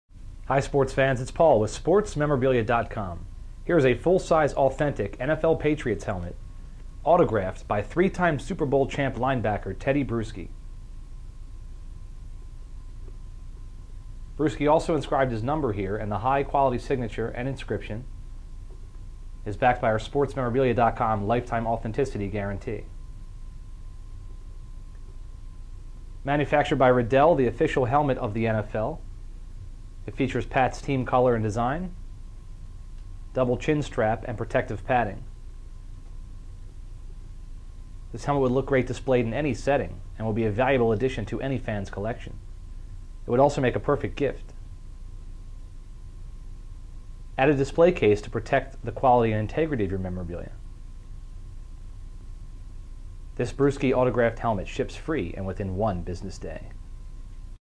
Tedy Bruschi Signed New England Patriots Authentic Helmet - SM Holo
This is an authentic helmet autographed by Pro Bowl linebacker, Tedy Bruschi. You can view more about this product The 3x Super Bowl Champ's bold, high quality signature is backed by our sportsmemorabilia.com lifetime authenticity guarantee. Manufactured by Riddell, the official helmet of the NFL, it features New England's team colors, double chin strap, and protective padding. This would make a great addition to any football fan's collection or a perfect gift. Add a display case to protect and preserve your memorabilia that features a removable glass top and mirrored bottom. This Tedy Bruschi autographed helmet ships free and within one business day.